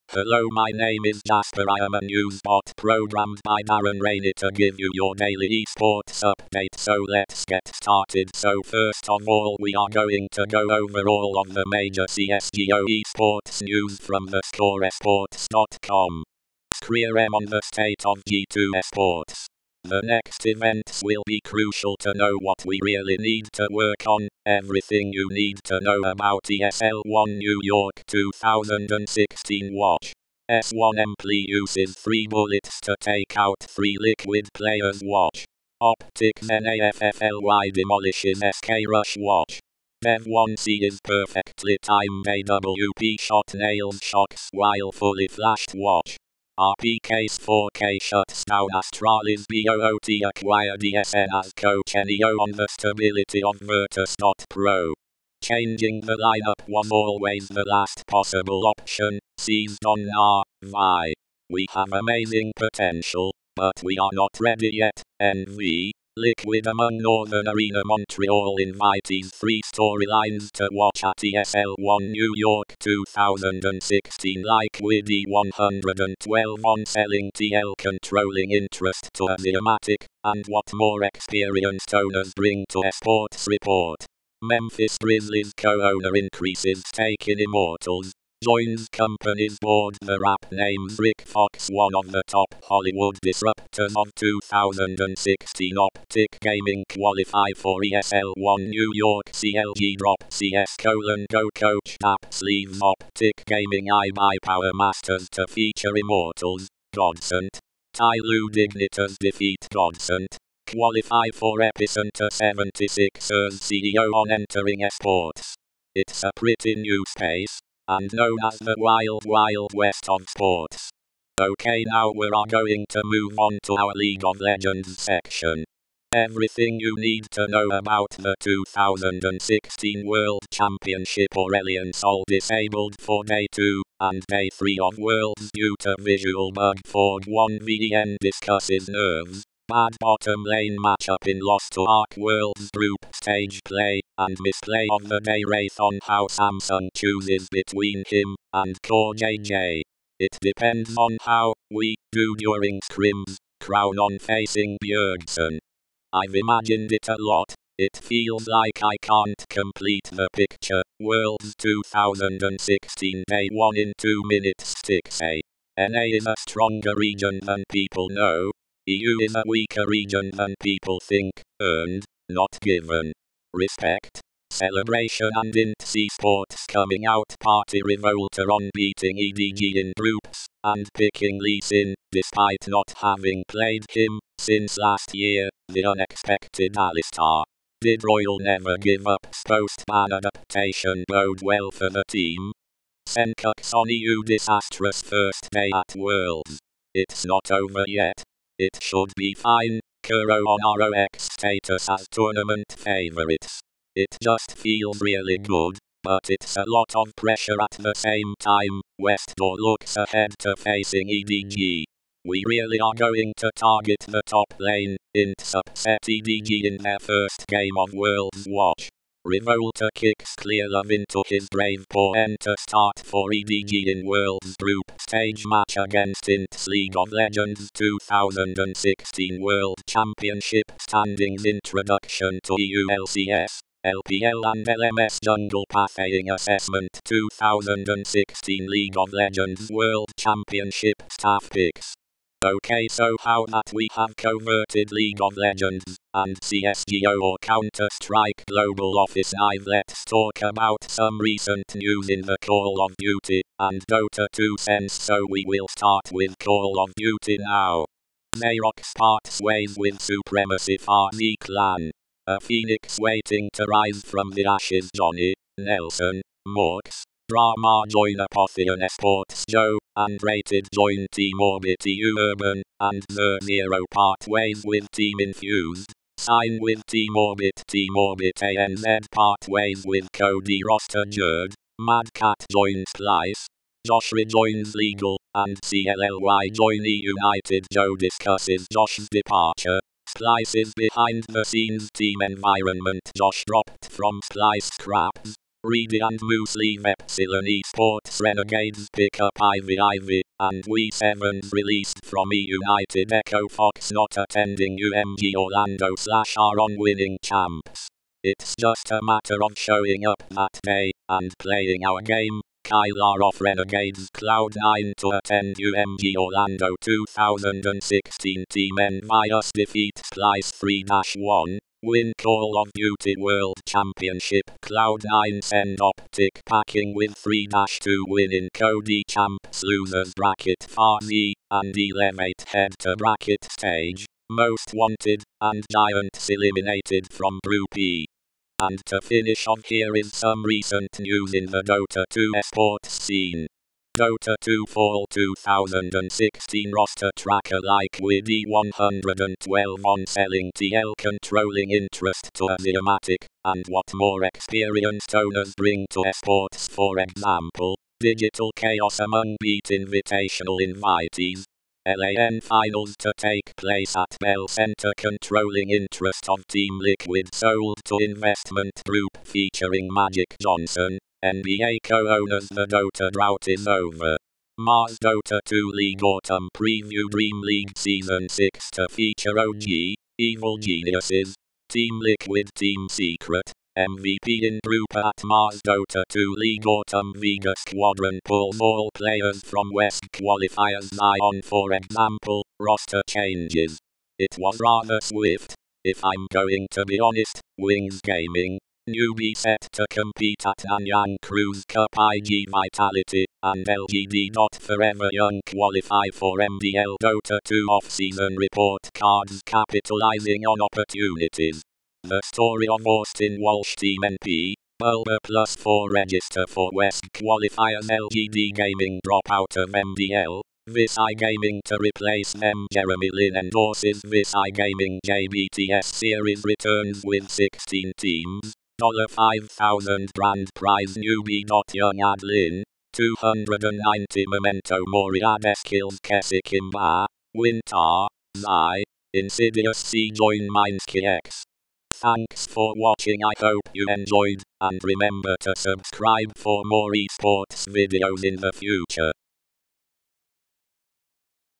Automated eSports News Report For Friday 30 of September 2016
ScreaM on the state of G2 Esports: 'The next events will be crucial to know what we really need to work on'
 Everything you need to know about ESL One New York 2016
 Watch: s1mple uses three bullets to take out three Liquid players
 Watch: OpTic's NAF-FLY demolishes SK's rush
 Watch: dev1ce's perfectly timed AWP shot nails shox while fully flashed
 Watch: RpK's 4k shuts down Astralis
 B.O.O.T acquire dsn as coach
 NEO on the stability of Virtus.pro: 'Changing the lineup was always the last possible option'
 seized on Na`Vi: 'We have amazing potential, but we are not ready yet'
 nV, Liquid among Northern Arena Montreal invitees
 3 storylines to watch at ESL One New York 2016
 LiQuiD112 on selling TL's controlling interest to aXiomatic and what more experienced owners bring to esports
 Report: Memphis Grizzlies co-owner increases stake in Immortals, joins company's board
 The Wrap names Rick Fox one of the top Hollywood disruptors of 2016
 OpTic Gaming qualify for ESL One New York
 CLG drop CS:GO coach
 daps leaves OpTic Gaming
 iBUYPOWER Masters to feature Immortals, GODSENT, TyLoo
 Dignitas defeat GODSENT, qualify for EPICENTER
 76ers CEO on entering esports: 'It's a pretty new space, and known as the Wild Wild West of sports'
 Everything you need to know about the 2016 World Championship
 Aurelion Sol disabled for Day 2 and Day 3 of Worlds due to visual bug
 FORG1VEN discusses nerves, bad bottom lane matchup in loss to ahq
 Worlds Group Stage Play and Misplay of the Day
 Wraith on how Samsung chooses between him and CoreJJ: 'It depends on how [we] do during scrims'
 Crown on facing Bjergsen: ‘I’ve imagined it a lot, it feels like I can’t complete the picture’
 Worlds 2016 Day 1 in two minutes
 Stixxay: 'NA is a stronger region than people know...EU is a weaker region than people think'
 Earned, not Given: Respect, Celebration and INTZ e-Sports’ coming-out party
 Revolta on beating EDG in groups and picking Lee Sin despite not having played him 'since last year'
 The unexpected Alistar: Did Royal Never Give Up's post-ban adaptation bode well for the team?
 Sencux on EU's disastrous first day at Worlds: 'It's not over yet...it should be fine'
 KurO on ROX's status as tournament favorites: 'It just feels really good, but it’s a lot of pressure at the same time'
 Westdoor looks ahead to facing EDG: 'We really are going to target the top lane'
 ​INTZ upset EDG in their first game of Worlds
 Watch: Revolta kicks Clearlove into his grave
 PawN to start for EDG in Worlds group stage match against INTZ
 League of Legends 2016 World Championship standings
 Introduction to EU LCS, LPL and LMS jungle pathing assessment
 2016 League of Legends World Championship Staff Picks
 Dota 2 Fall 2016 Roster Tracker
 EG, Digital Chaos among BEAT Invitational invitees; LAN Finals to take place at Bell Centre
 Controlling interest of Team Liquid sold to investment group featuring Magic Johnson, NBA co-owners
 The Dota drought is over: Mars Dota 2 League Autumn Preview
 DreamLeague Season 6 to feature OG, Evil Geniuses, Team Liquid
 Team Secret, MVP in Group A at Mars Dota 2 League Autumn
 Vega Squadron pulls all players from WESG qualifiers
 zai on EG' roster changes: 'It was rather swift, if I'm going to be honest'
 Wings Gaming, Newbee set to compete at Nanyang Cruise Cup
 iG Vitality and LGD.Forever Young qualify for MDL
 Dota 2 offseason report cards
 Capitalizing on Opportunities: The Story of Austin Walsh
 Team NP, Bulba + 4 register for WESG qualifiers
 LGD Gaming drop out of MDL, Vici Gaming to replace them
 Jeremy Lin endorses Vici Gaming J
 BTS Series returns with 16 teams, $5,000 grand prize
 Newbee.Young add Lin, 290
 Memento Mori add Eskillz
 Kecik Imba, WinteR, zai`, InsidiousC join Mineski-X
 Zayrox parts ways with Supremacy
 FaZe Clan: A phoenix waiting to rise from the ashes
 Johnny, Nelson, Maux, DraMa join A